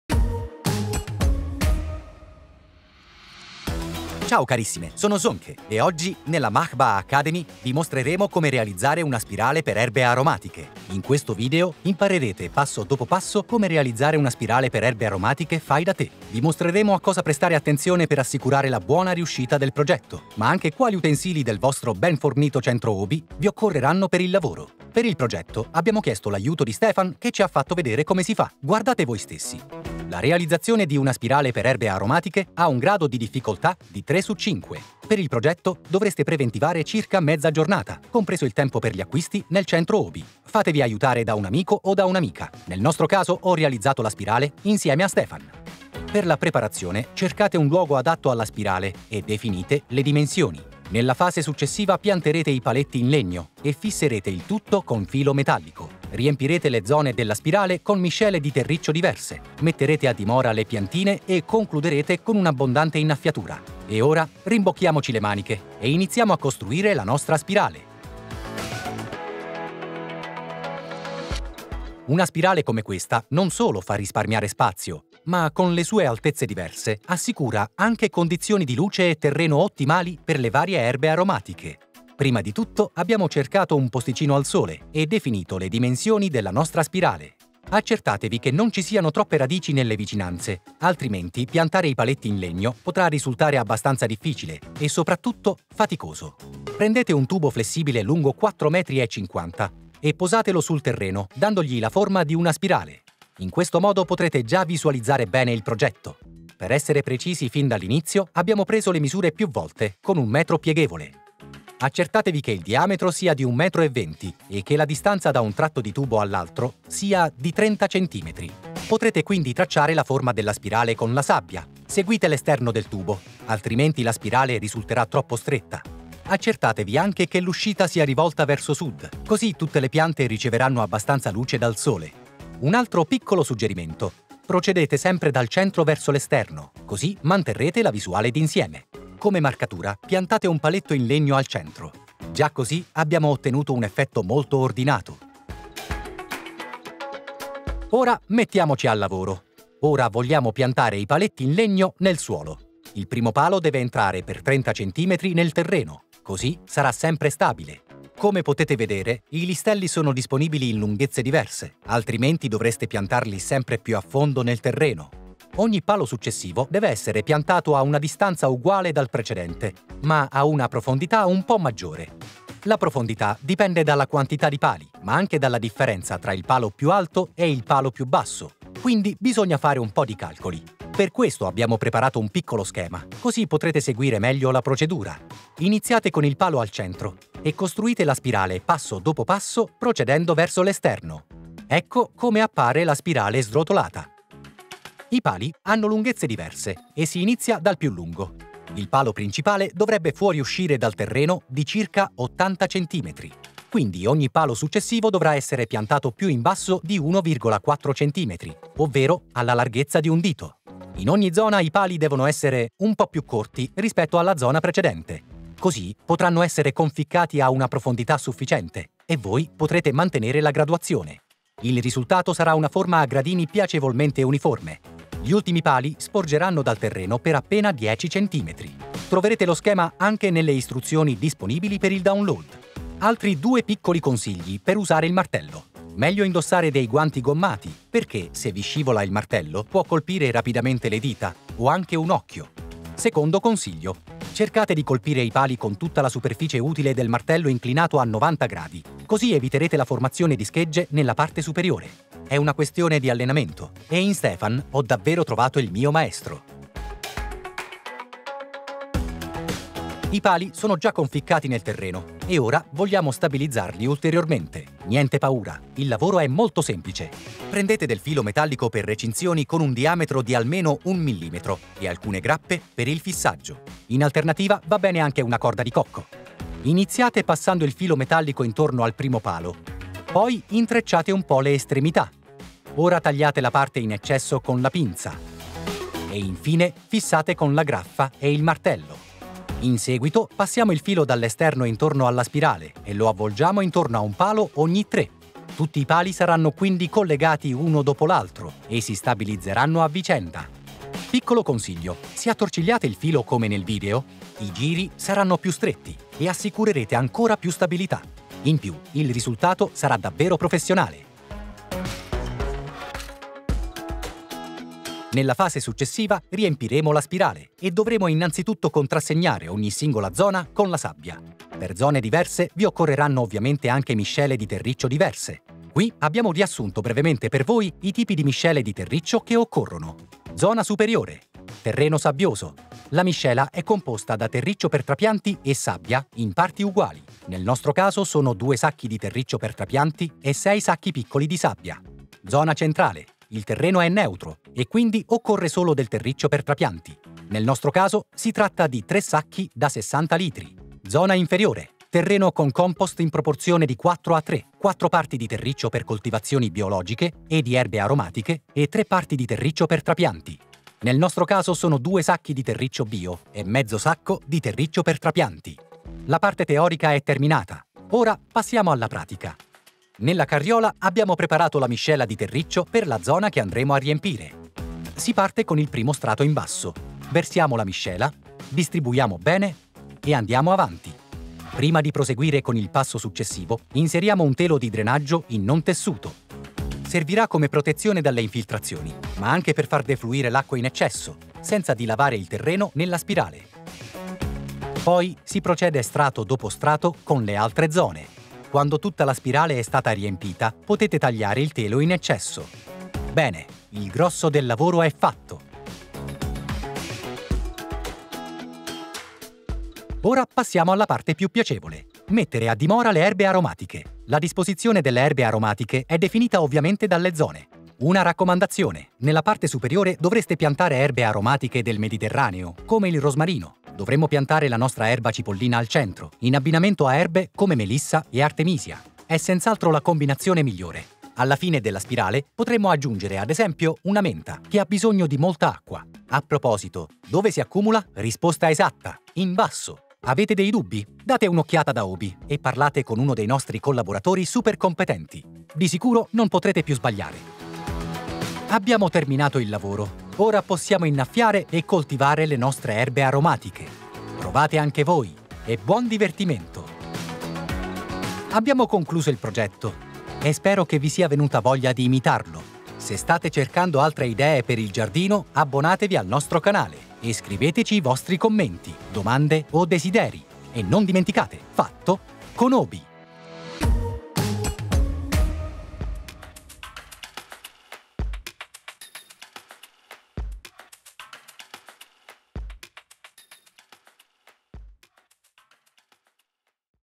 Costruire una spirale per erbe aromatiche – con paletti in legno| OBI
Costruire una spirale per offrire alle erbe aromatiche terreno e condizioni di luce ideali? Con un po’ di abilità ci riuscirai senza problemi. Nel nostro video Sönke e il giardiniere diplomato Stefan ti spiegano come puoi progettare e costruire una spirale per erbe aromatiche, ma anche come mettervi ottimamente a dimora le piante.